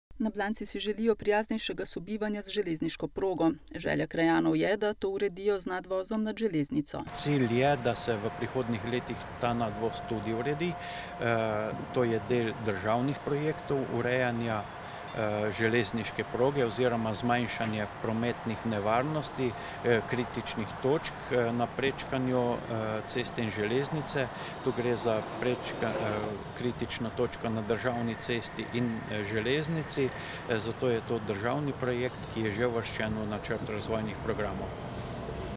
Kdaj na Blanci nadvoz nad železniško progo?
🚉 Kdaj na Blanci nadvoz nad železniško progo? Pravijo, da bo! Nadvoz je državni projekt urejanja železniške proge oz. zmanjšanja prometnih nevarnosti, kritičnih točk na prečkanju ceste in železnice, o katerem govorijo že dolgo časa.